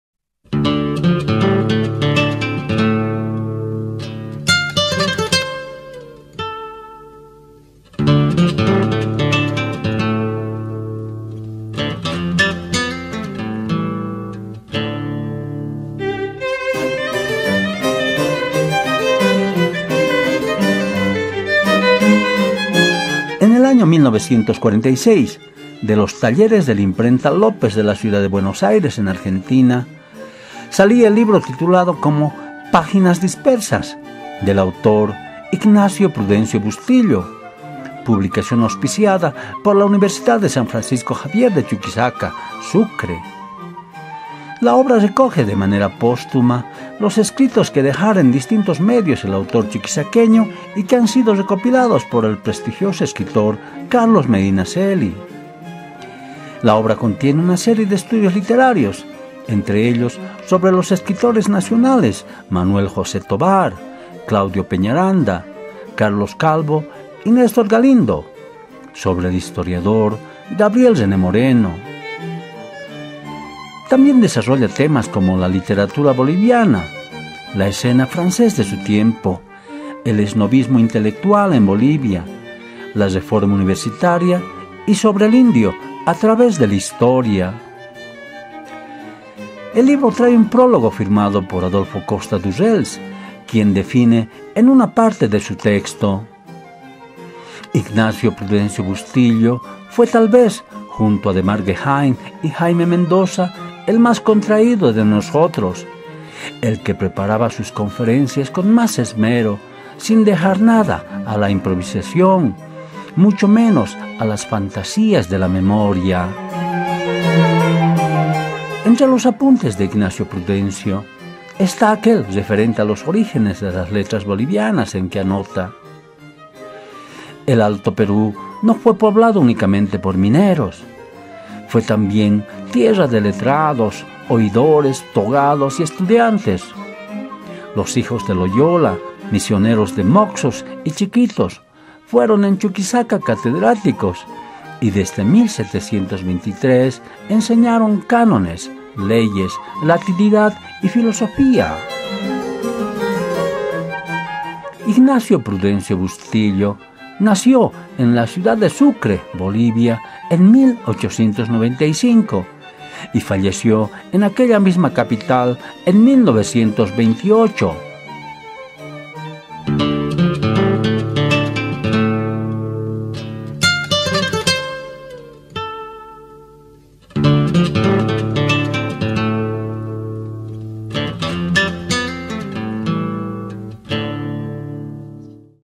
PÁGINAS DISPERSAS (1946) de Ignacio Prudencio Bustillo
Libro que recoge textos recopilados por Carlos Medinaceli. Ignacio Prudencio Bustillo (Sucre, Bolivia, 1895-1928). Producción del Museo del Aparapita.